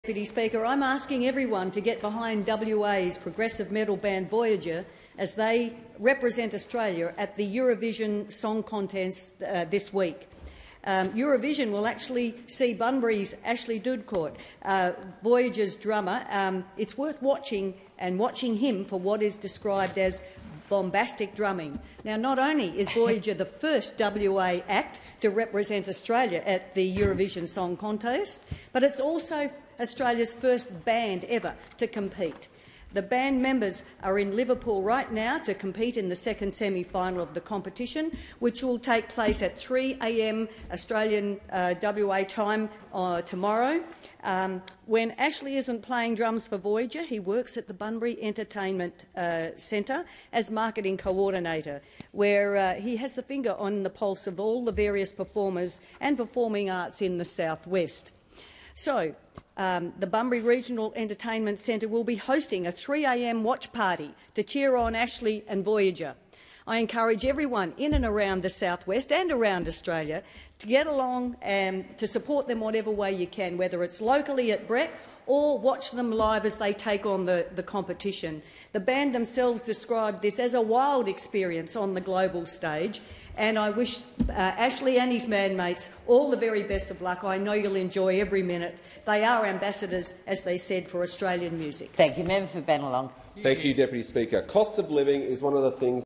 Eurovision Song Contest - Good luck Voyager!
I'm asking everyone to get behind WA progressive metal band Voyager as they represent Australia at the Eurovision song contest on Friday 12th May 2023. Eurovision will see Bunbury's Ashley Doodkorte play as Voyager's drummer!